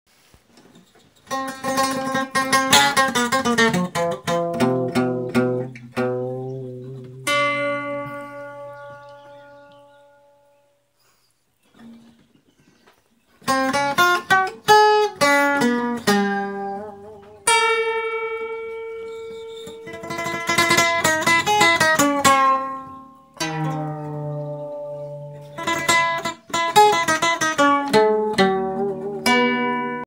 Traditional Egyptian Gypsy Folk Song (preview)
Father & Son
Traditional Egyptian Gypsy Folk Song for Barn Harp and Farm Guitar (preview)

Janson Michael Steffan - farm guitar
William Janson Marohl-Steffan - barn harp